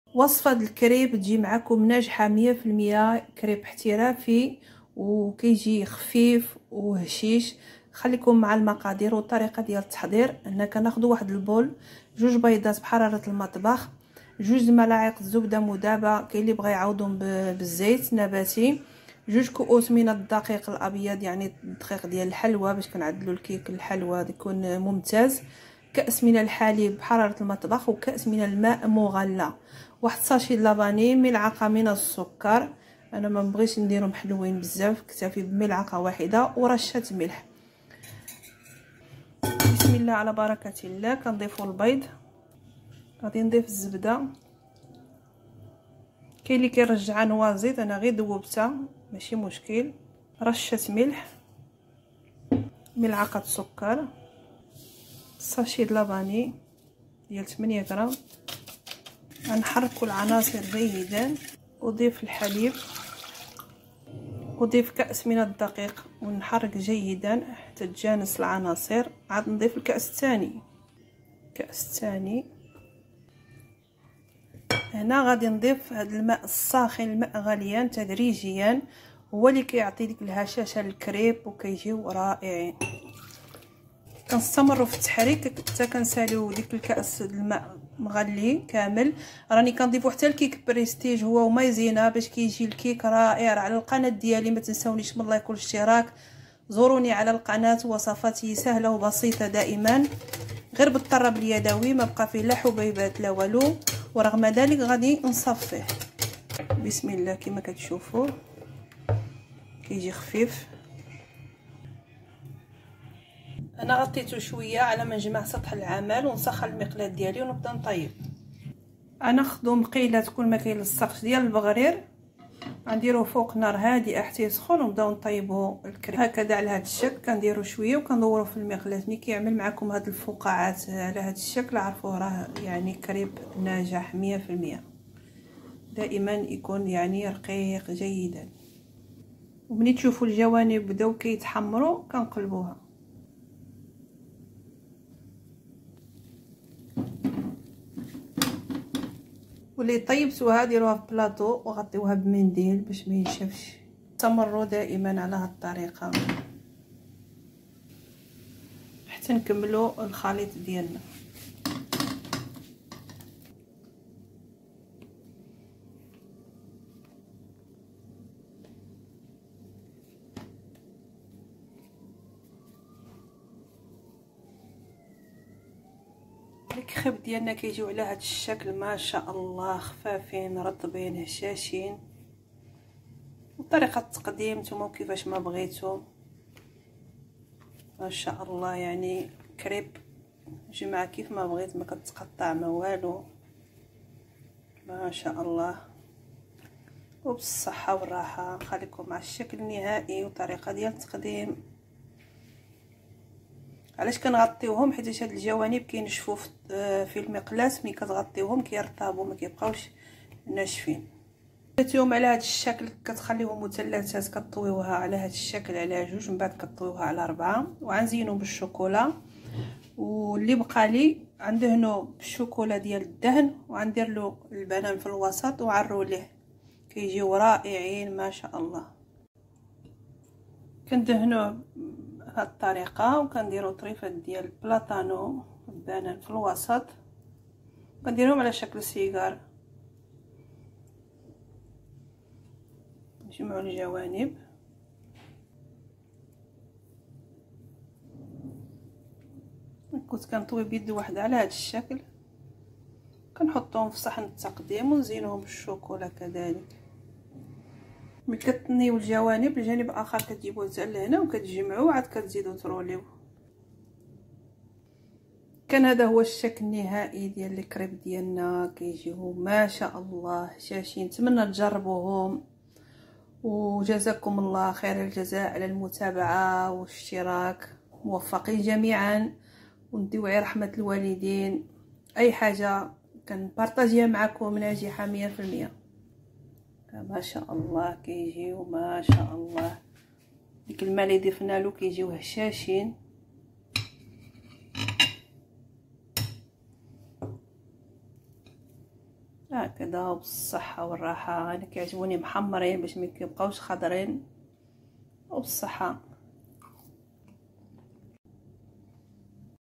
كريب ناجح 💯👌هشيش وبنين كيحبوه الأطفال رائع جدا
السلام عليكم ورحمة الله وبركاته موفقين مرحبا بالجميع في قناتي المتواضعه متابعه ولايك مردودة لكم👍🤗⚘️

كريب خفيف وبنين كيجي هشيش ياسلام
كريب بالشوكولاته والموز
ناجح 💯💯💯
طريقة سهلة وبسيطة 👍🥰🥰